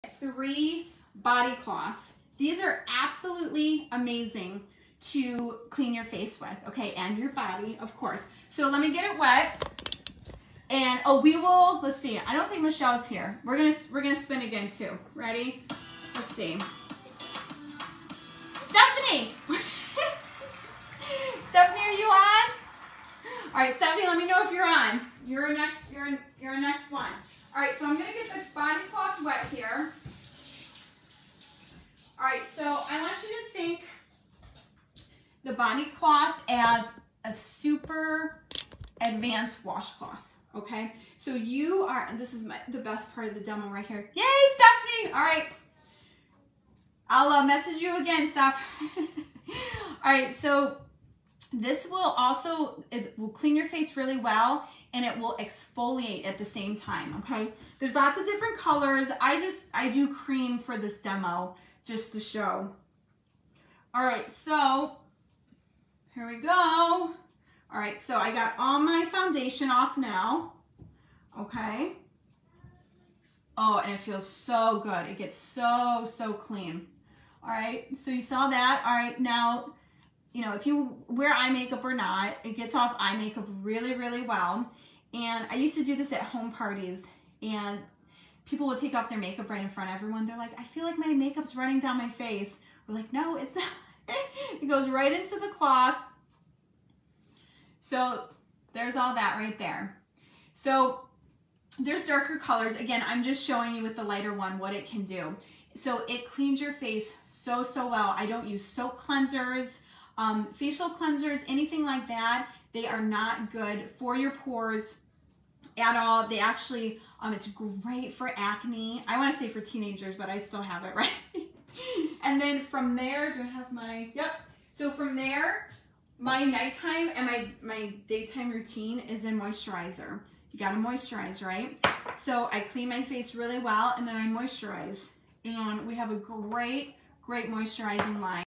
The Amazing Face & Body Cloth!
✨𝗕𝗢𝗗𝗬 & 𝗙𝗔𝗖𝗘 𝗣𝗔𝗖𝗞!!✨

Did you know? Norwex has AMAZING bath and body products as well?

If you buy eye make-up remover, make-up remover wipes, soap, shaving cream, facial cleansers or shower gel...time to listen up!! 🤯You can replace ALL of that with our body cloth 👏🏼👏🏼

✨Body cloths are one of my absolute favorites! Who has one? 🙋🏼‍♀️They are like a super advanced washcloth 👏🏼👏🏼

I use them to wash my face at night (takes off makeup easily!) and in the morning, plus have one in the shower.

No soap required! 😳 🧼 Facial cleaners can actually clog up your pores? 😢

If you do research, cleaning is really just removing dead skin cells and Body Cloths exfoliate so well! They grab everything out of those pores.

Best thing is then rinse it, hang up and it’s ready to go for the next day.

Great for acne as well!

Also love it for kids in the bath and I’m not using a different washcloth every day. 🙌🏼

Can’t go wrong with Body Cloths!

𝙏𝙝𝙞𝙨 𝙞𝙨 𝙤𝙣𝙚 𝙤𝙛 𝙢𝙮 𝙩𝙤𝙥 5 𝙛𝙖𝙫𝙤𝙧𝙞𝙩𝙚 𝙥𝙧𝙤𝙙𝙪𝙘𝙩𝙨!